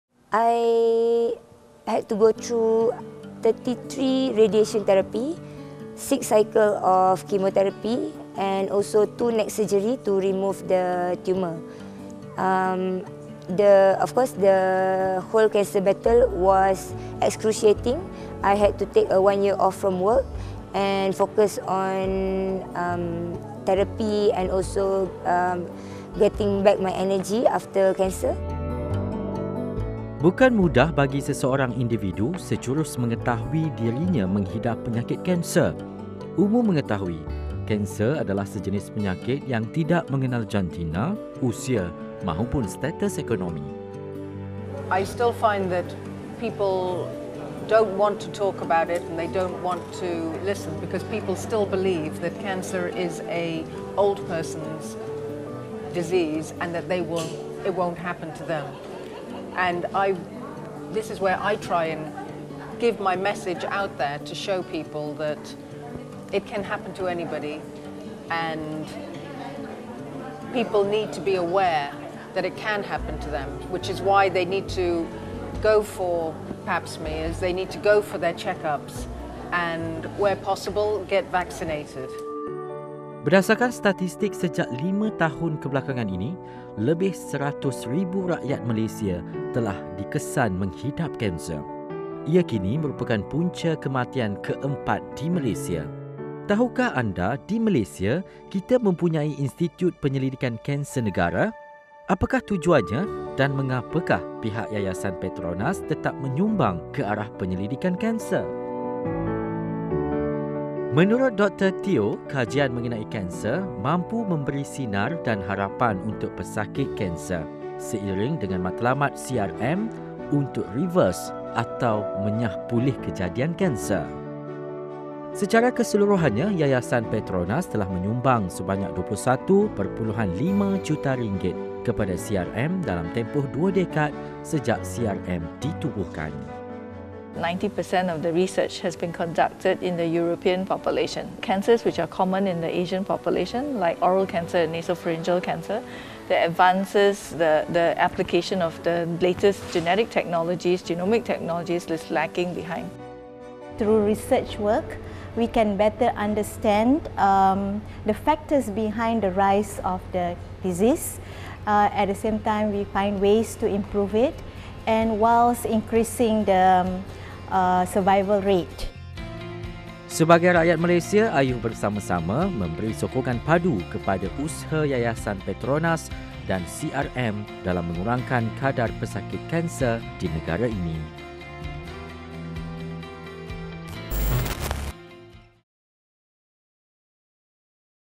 AWANIByte: Yayasan PETRONAS komited bantu CRM jalani penyelidikan untuk penyakit kanser di Asia
Sejak penubuhannya pada awal 2001, Canser Research Malaysia sering dibantu Yayasan PETRONAS dari segi dana dan kerjasama strategik dalam menjalankan kajian khusus bagi mengenal pasti punca utama penyakit kanser, khususnya di kalangan orang Asia.